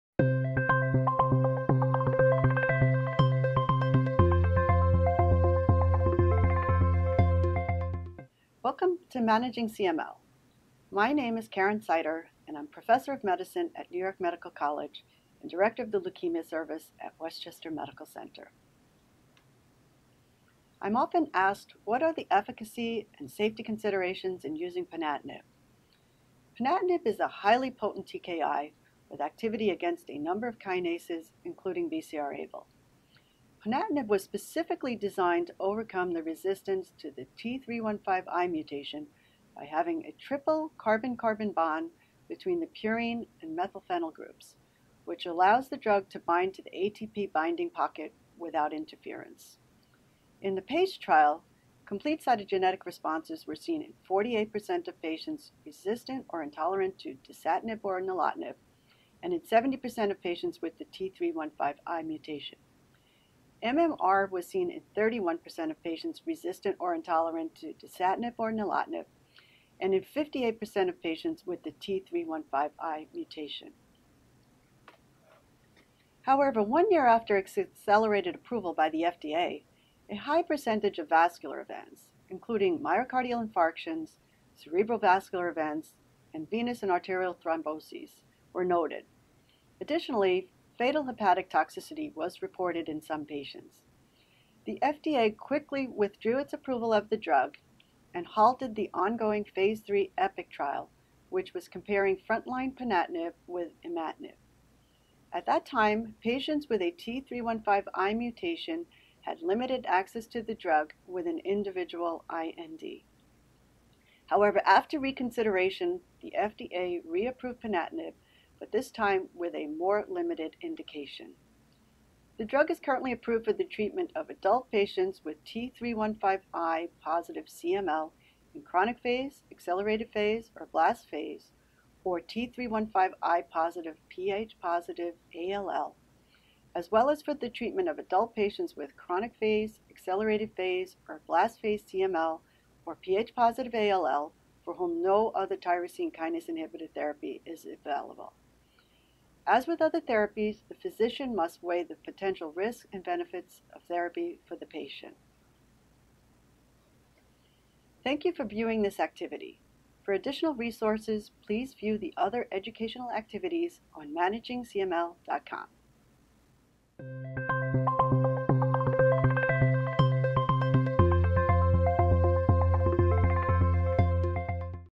What are the efficacy and safety considerations in using ponatinib?
Dr. Karen Seiter discusses the efficacy and safety considerations in using ponatinib.

Join our online communities to begin receiving free continuing medical education (CME) credit today!